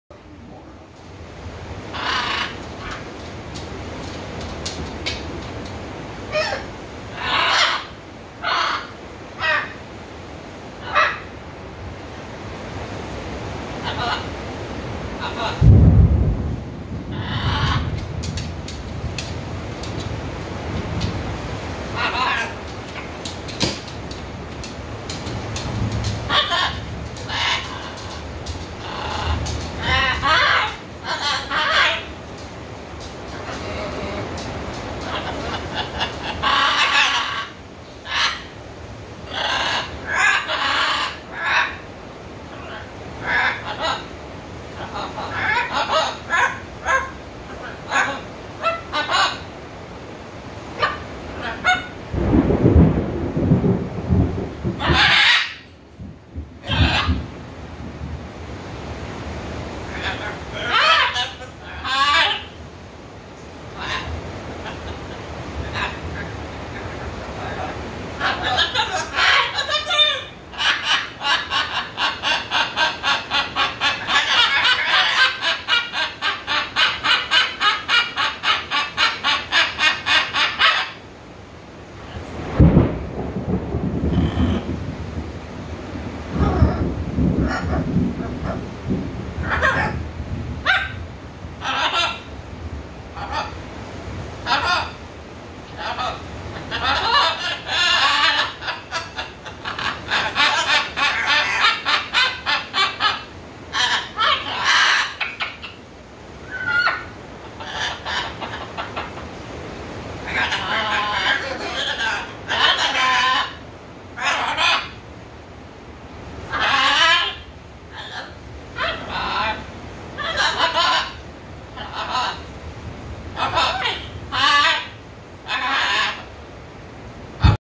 Jack and Star talking while it's raining
Rain, Macaw, Parrot, talking, birds